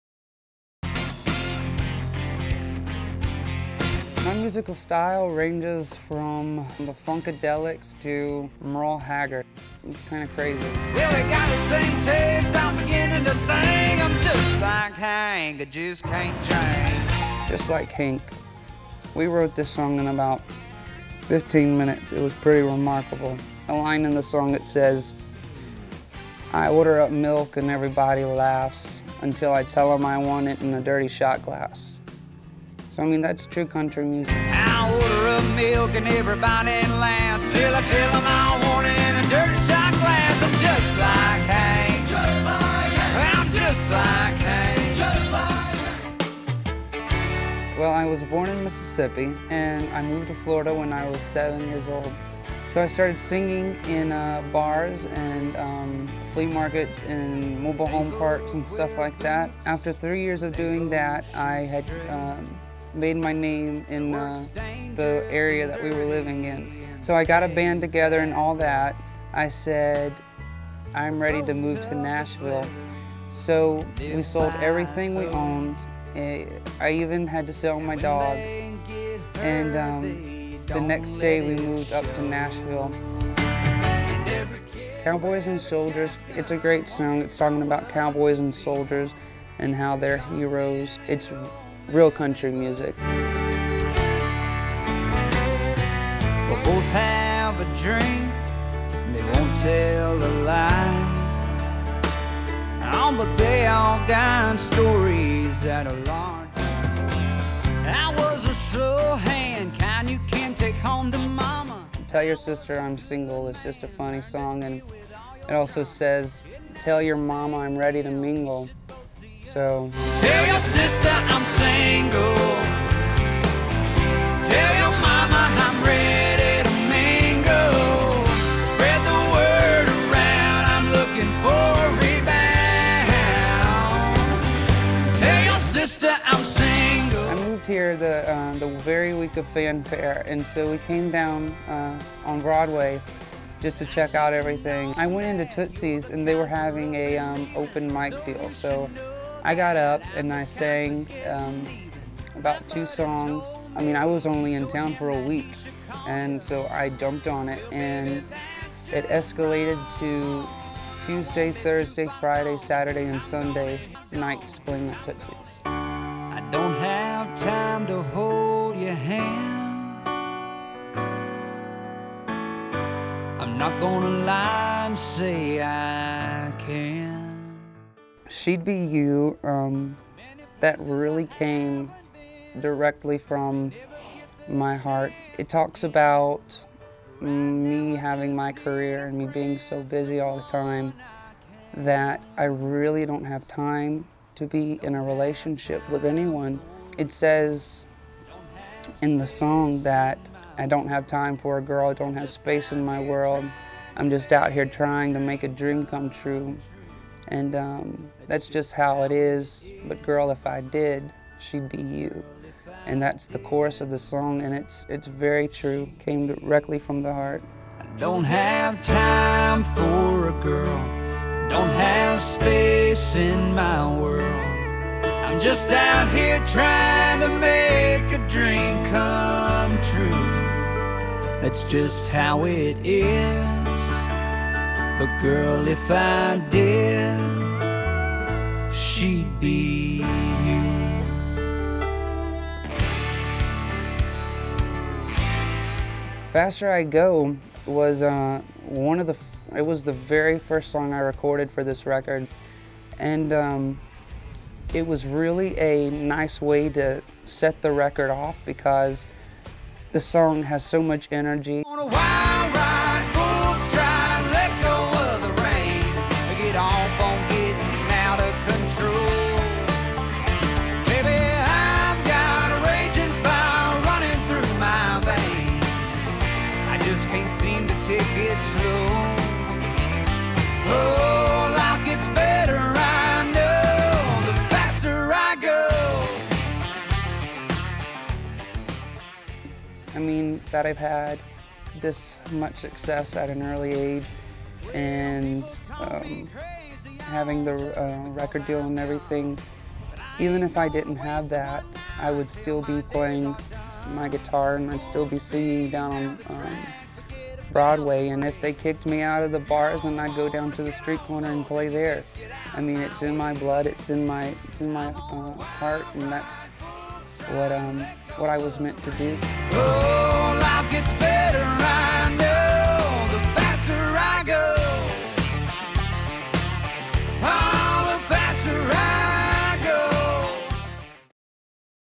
Tyler Dickerson - Who is Tyler Dickerson? (HD)
Tyler Dickerson on the story behind his songs.